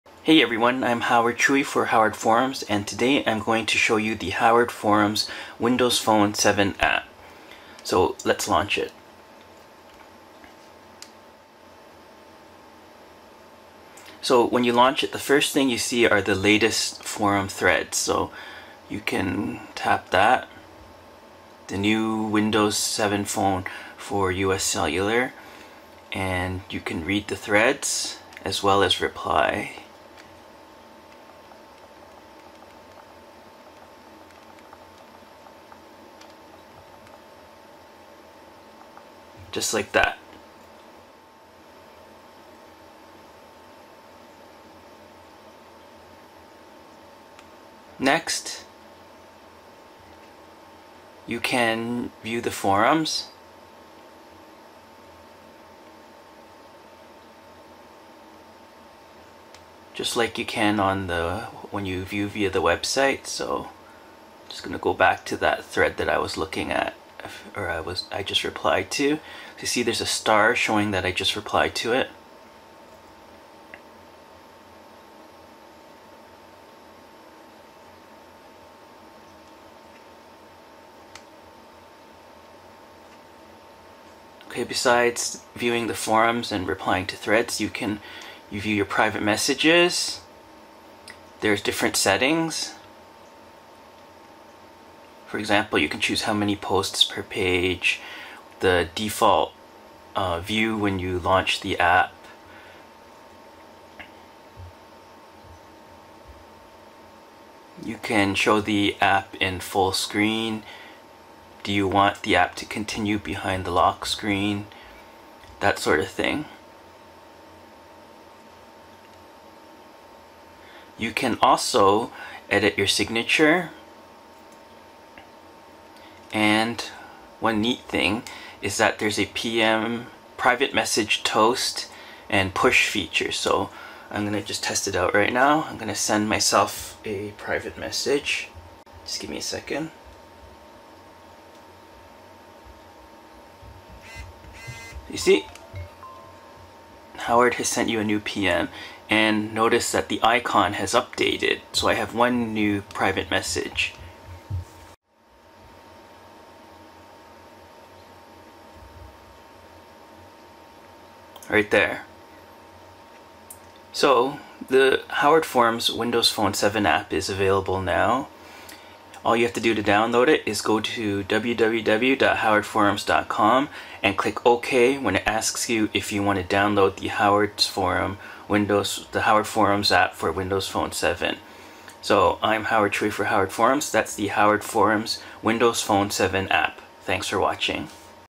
Introducing the HowardForums app for Windows Phone 7
Here's the HowardForums app for Windows Phone 7. With it you can:

view the latest threads
browse the forums
reply to and post new threads
check/reply/send private messages
search
etc

It also has private messaging toast notifications so you can see how many new pm's you have from the home screen plus be notified when you get a new one.